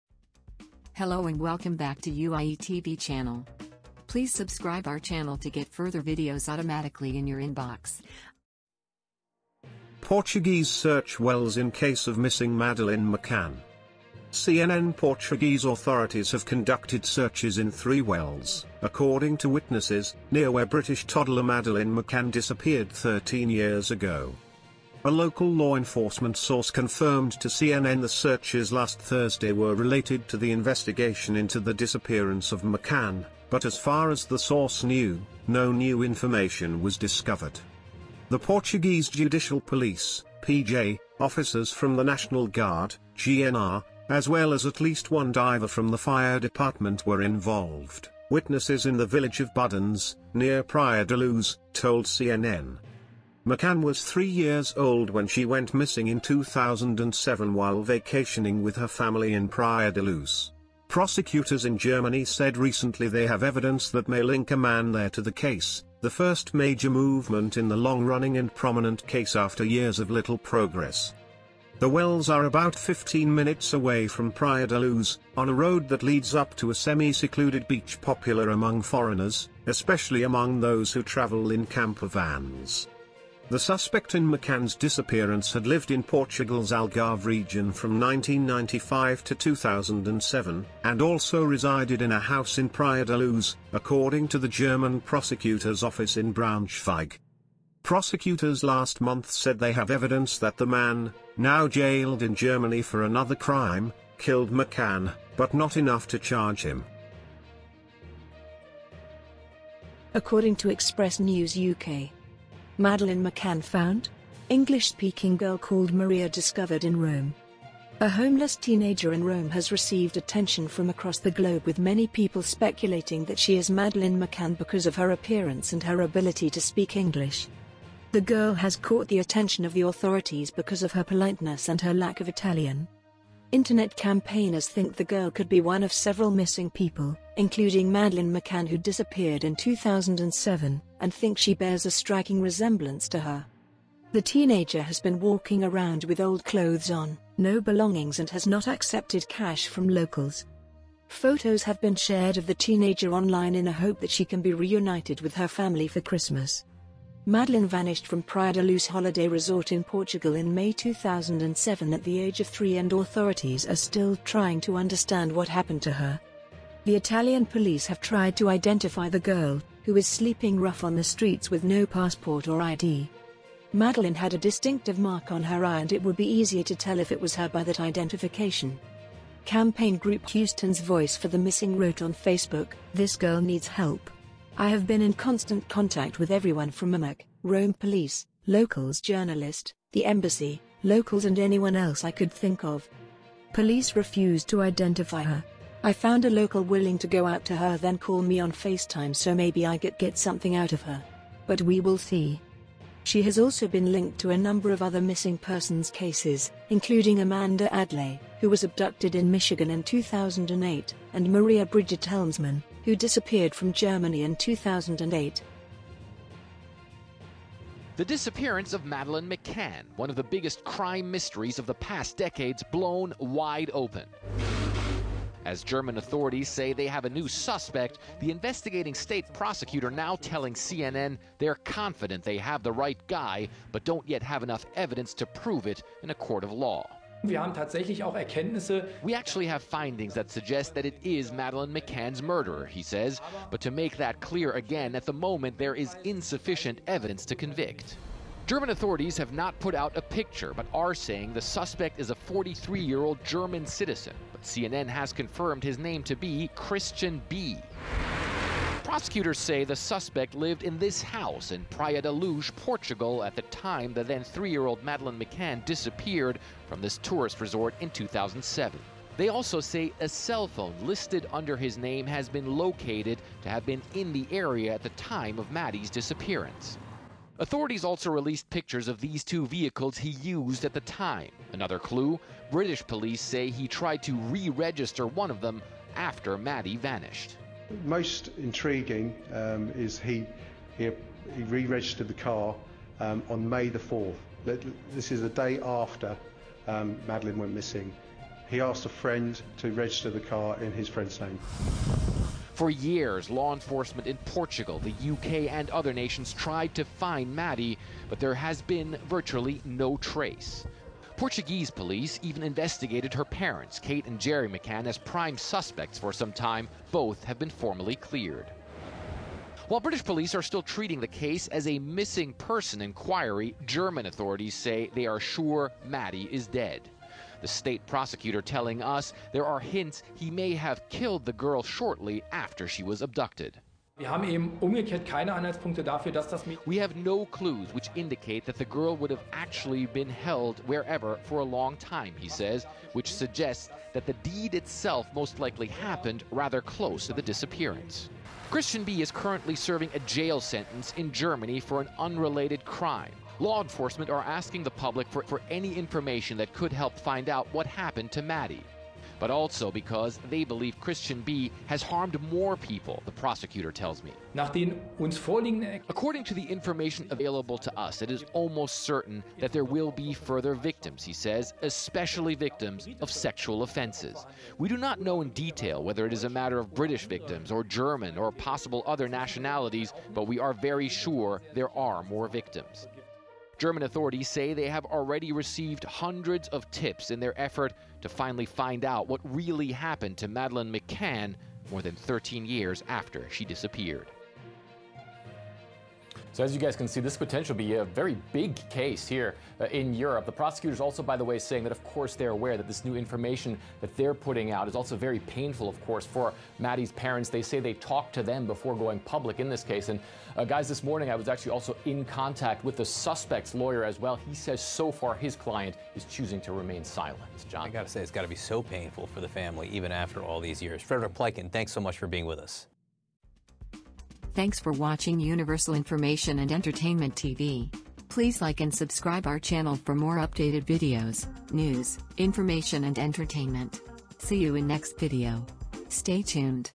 Missing Madeleine McCann FOUND? English speaking girl called 'Maria' discovered in Rome
Portuguese search wells in case of missing Madeleine McCann.

(CNN)Portuguese authorities have conducted searches in three wells, according to witnesses, near where British toddler Madeleine McCann disappeared 13 years ago.

A local law enforcement source confirmed to CNN the searches last Thursday were related to the investigation into the disappearance of McCann, but as far as the source knew, no new information was discovered.
The Portuguese Judicial Police (PJ), officers from the National Guard (GNR) as well as at least one diver from the fire department were involved, witnesses in the village of Budens, near Praia da Luz, told CNN.
McCann was 3 years old when she went missing in 2007 while vacationing with her family in Praia da Luz. Prosecutors in Germany said recently they have evidence that may link a man there to the case, the first major movement in the long-running and prominent case after years of little progress.

The wells are about 15 minutes away from Praia da Luz, on a road that leads up to a semi-secluded beach popular among foreigners, especially among those who travel in camper vans.
The suspect in McCann's disappearance had lived in Portugal's Algarve region from 1995 to 2007, and also resided in a house in Praia de Luz, according to the German prosecutor's office in Braunschweig.
Prosecutors last month said they have evidence that the man, now jailed in Germany for another crime, killed McCann, but not enough to charge him.

according to express news UK.

Madeleine McCann FOUND? English-speaking girl called 'Maria' discovered in Rome.
A HOMELESS teenager in Rome has received attention from across the globe with many people speculating that she is Madeleine McCann because of her appearance and her ability to speak English.

The girl has caught the attention of the authorities because of her politeness and her lack of Italian.

Internet campaigners think the girl could be one of several missing people, including Madeleine McCann who disappeared in 2007, and think she bears a striking resemblance to her.

The teenager has been walking around with old clothes on, no belongings and has not accepted cash from locals.
Photos have been shared of the teenager online in a hope that she can be reunited with her family for Christmas.

Madeleine vanished from Praia da Luz holiday resort in Portugal in May 2007 at the age of three and authorities are still trying to understand what happened to her.

The Italian police have tried to identify the girl, who is sleeping rough on the streets with no passport or ID.
“Madeleine had a distinctive mark on her eye and it would be easy to tell if it was her by that identification.”

Campaign group Houston's Voice For The Missing wrote on Facebook: "This girl needs help. I have been in constant contact with everyone from NCMEC, Rome police, locals journalist, the embassy, locals and anyone else I could think of.

"Police refuse to identify her. But we will see.”

She has also been linked to a number of other missing persons cases, including Amanda Adlai, who was abducted in Michigan in 2008, and Maria-Brigitte Helmsman, who disappeared from Germany in 2008.